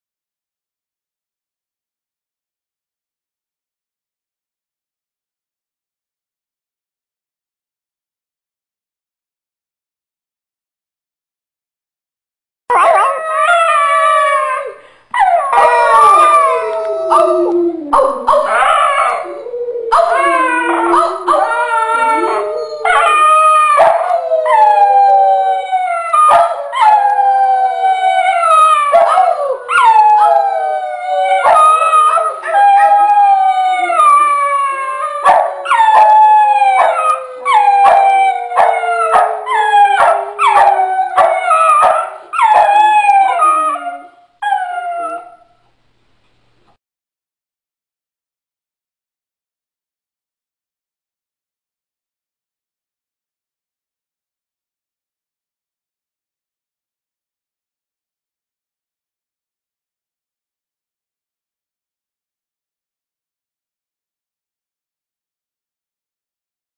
"Howling for Hillary"
Starring Millie and Maddie. Howling and Singing their love and support for Hillary Clinton.